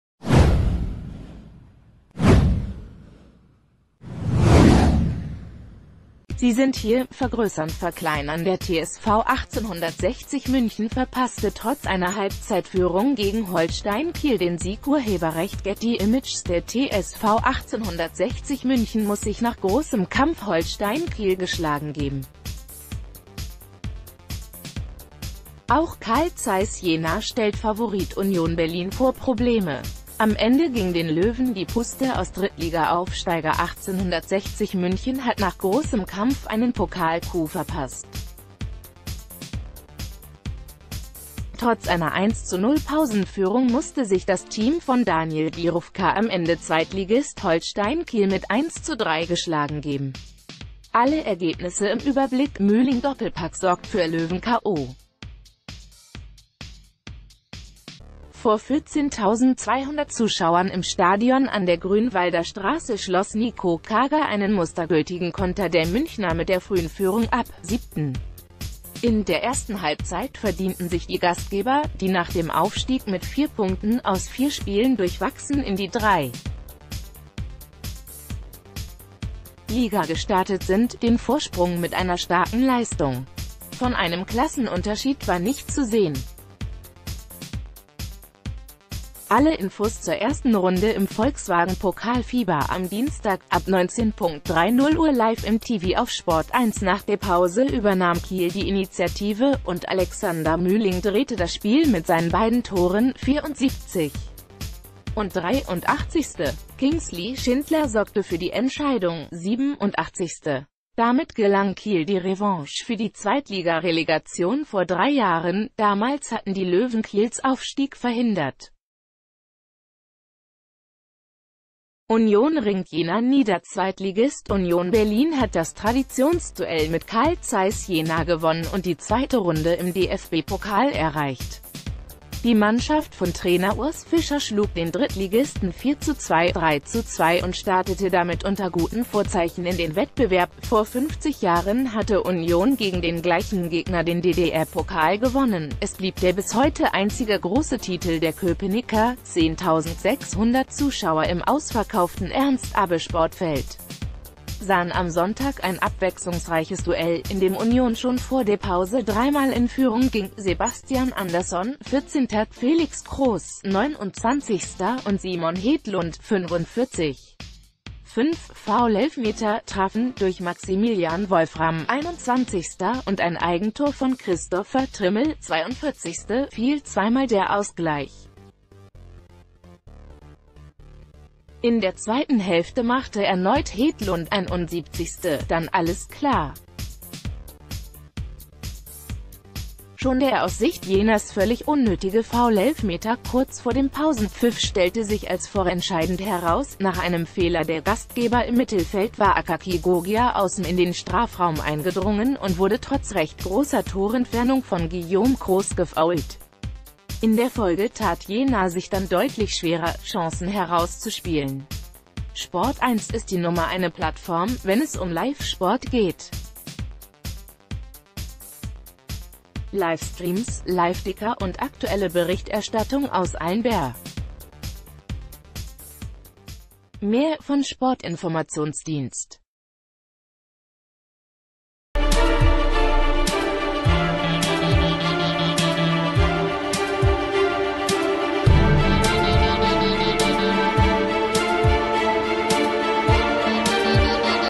DFB-Pokal: 1. Runde mit TSV 1860 München und Union Berlin
Sie sind hier: vergrößernverkleinern Der TSV 1860 München verpasste trotz einer Halbzeitführung gegen Holstein Kiel den Sieg © Getty Images Der TSV 1860 München muss sich nach großem Kampf Holstein Kiel geschlagen geben. Auch Carl Zeiss Jena stellt Favorit Union Berlin vor Probleme. Am Ende ging den Löwen die Puste aus: Drittliga-Aufsteiger 1860 München hat nach großem Kampf einen Pokalcoup verpasst. Trotz einer 1:0-Pausenführung musste sich das Team von Daniel Bierofka am Ende Zweitligist Holst...